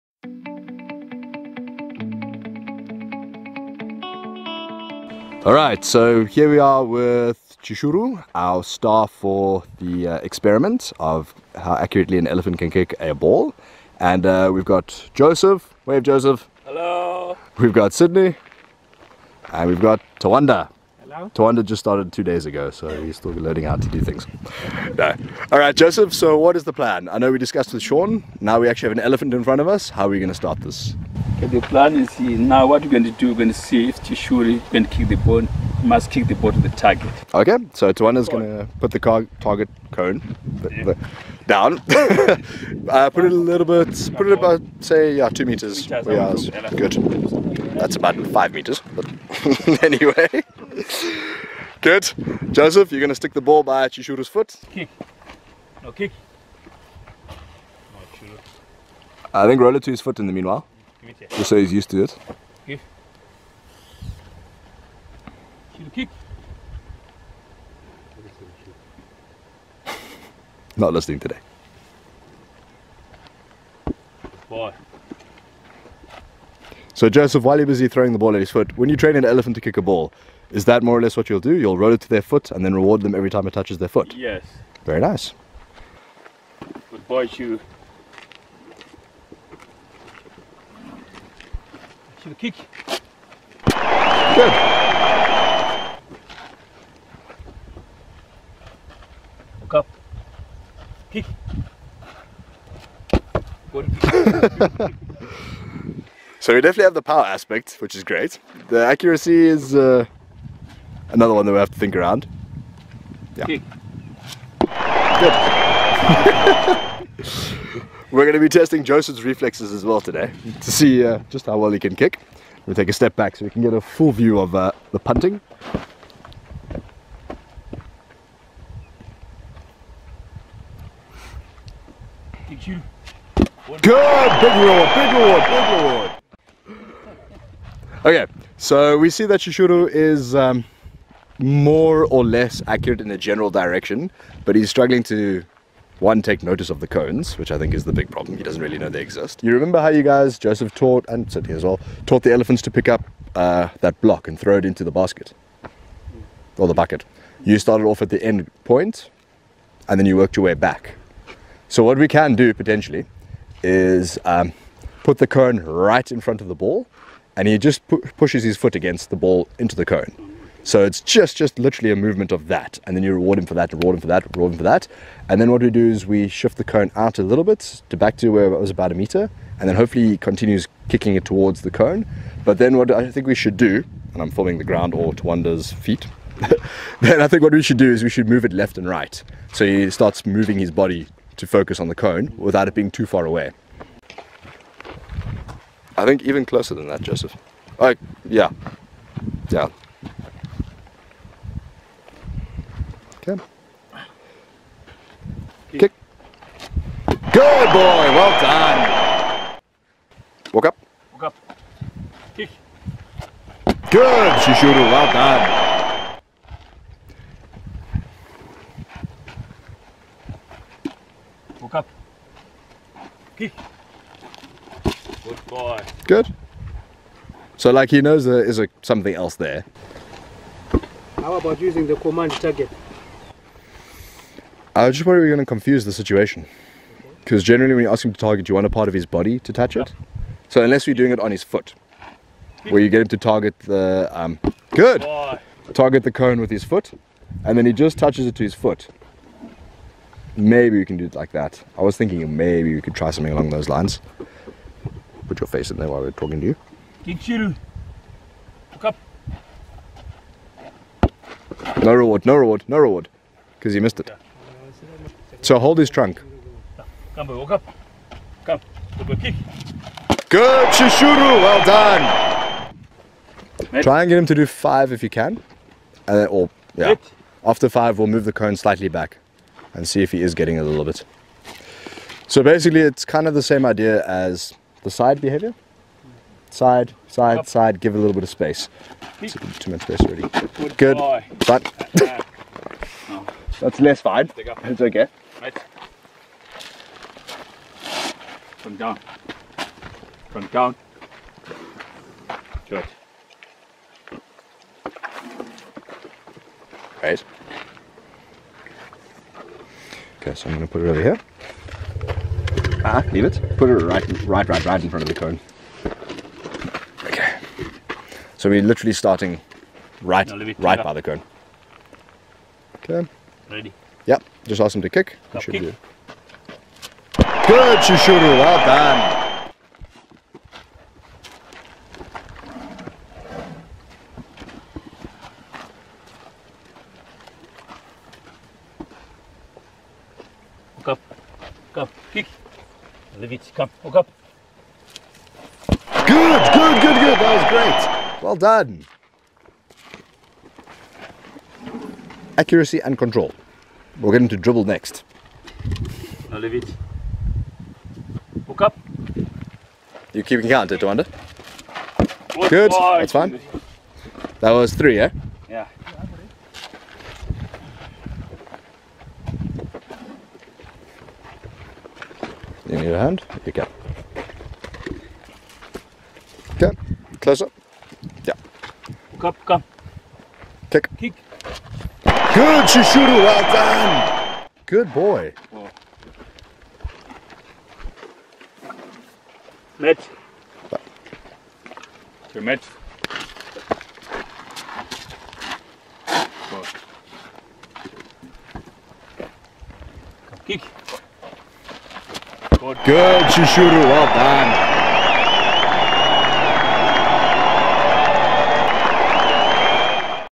The Elephant Hypothesis Episode 3
Can an Elephant kick a ball at a target